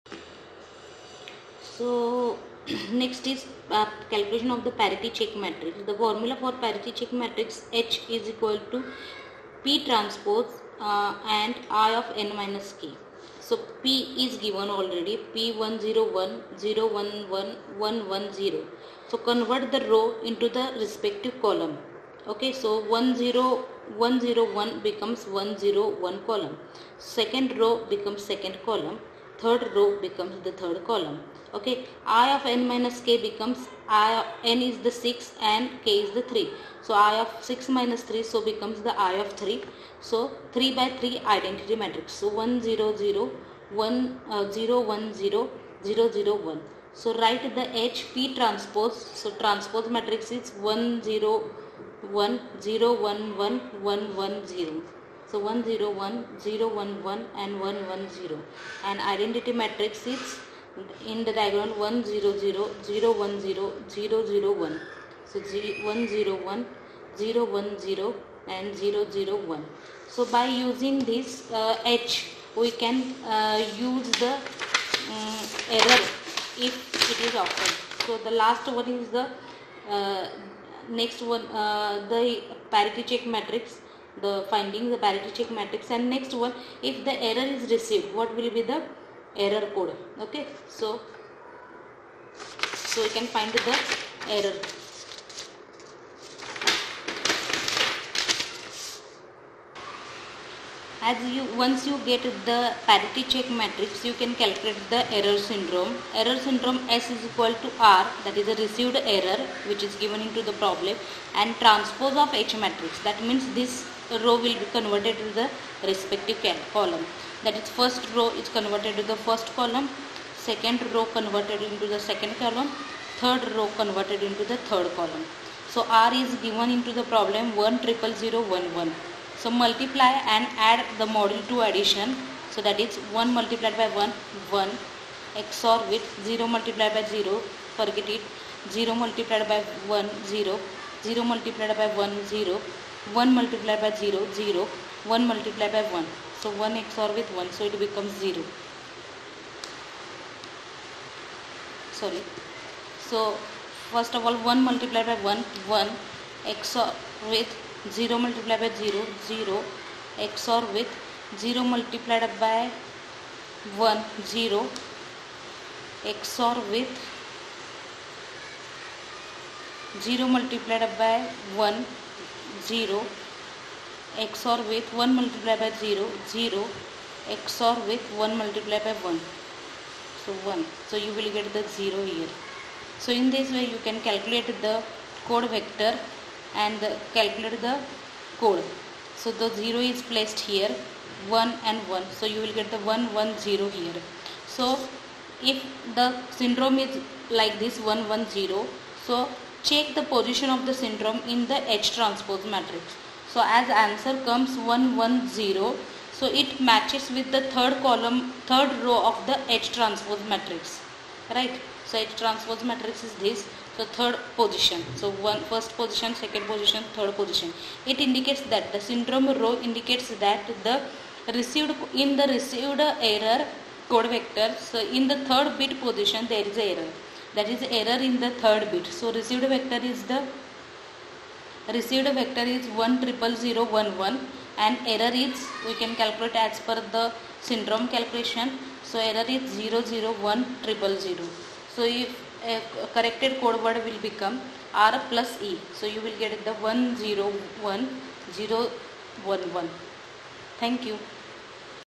Linar block coding-calculation parity check matrix and syndrome
Ms.S.Y.Salunkhe
Subject:-Information theory and coding
Chapter no:-4 error detecting and correcting codes
Contents-Parity check matrix